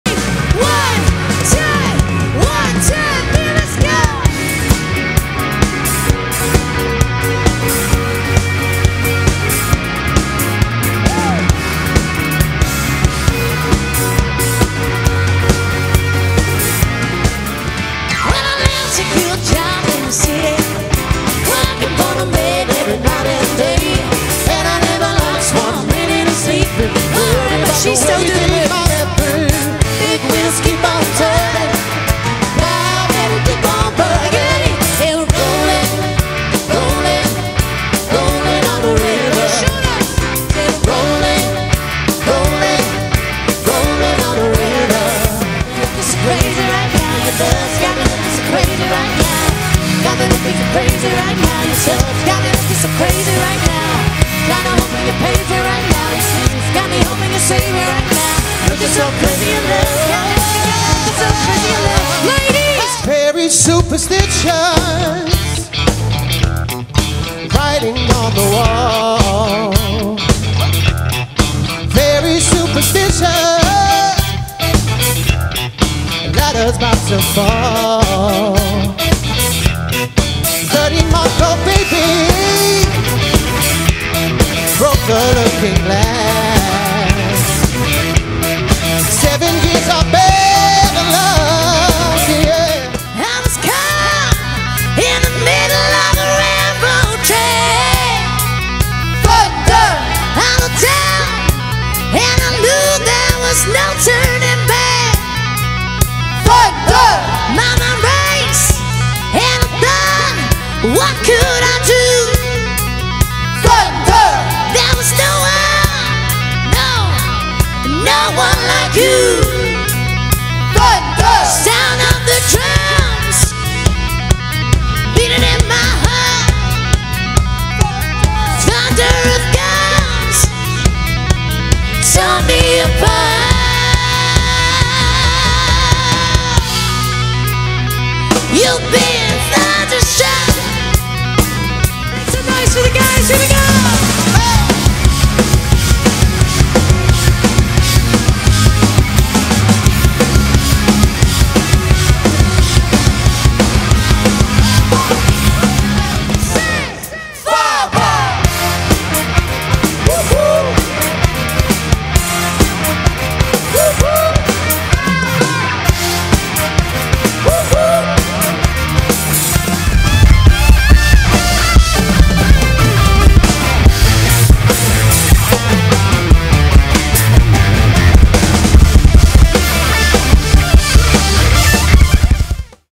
The Front -Full Showreel- AliveNetwork.com
The Front are one of the UK's biggest event bands! We're known for our signature high energy set and stage performance full of contemporary and dance floor classics, along with some old school mash-ups guaranteed to get your guests dancing.